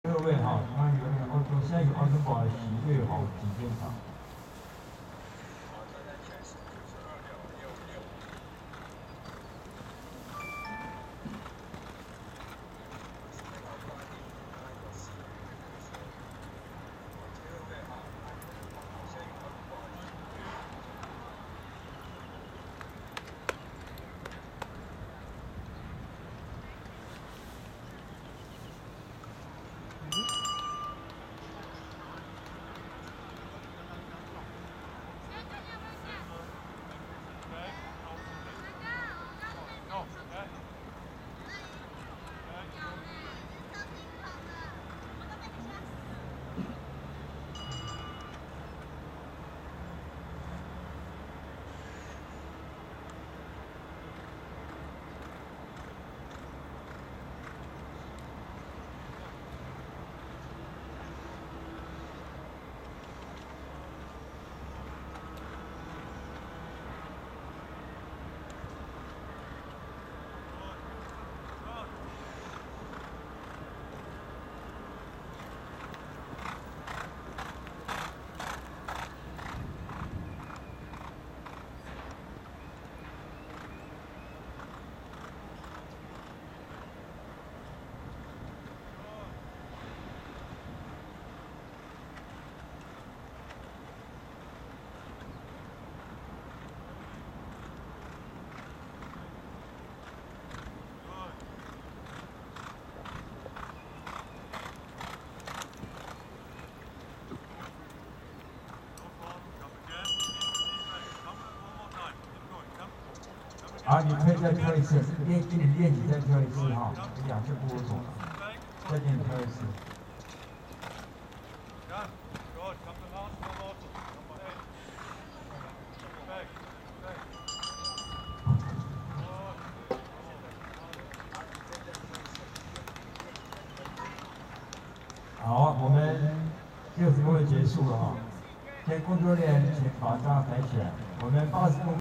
2021年FEI世界挑戰盃第三場暨全國障礙超越錦標賽(積分第4站) 60CM組 許睿恆
比賽地點：澳登堡馬術指導中心
馬名：亞歷山大
代表馬場：澳登堡馬術指導中心